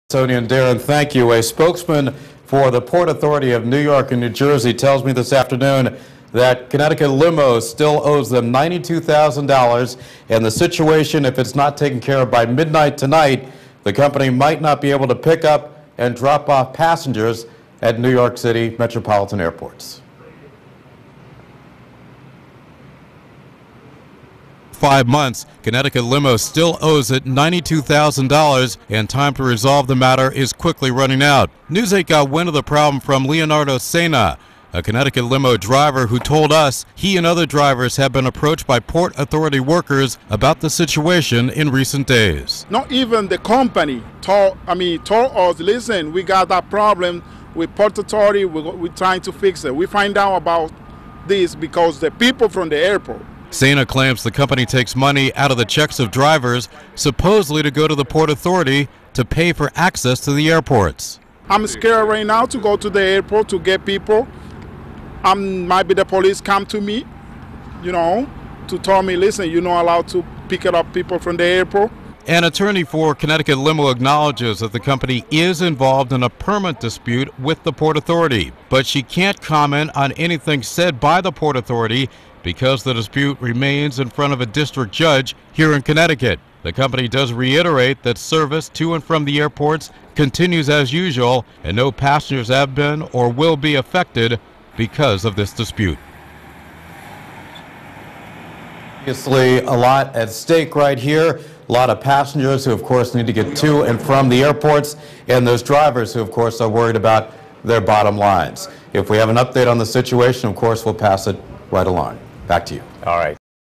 Port Authority: CT Limo owes $92,000
Connecticut Limos ability to shuttle passengers to airports in the New York City metropolitan area is in jeopardy, as the Port Authority is threatening to ban the company's vehicles because of an unpaid tab.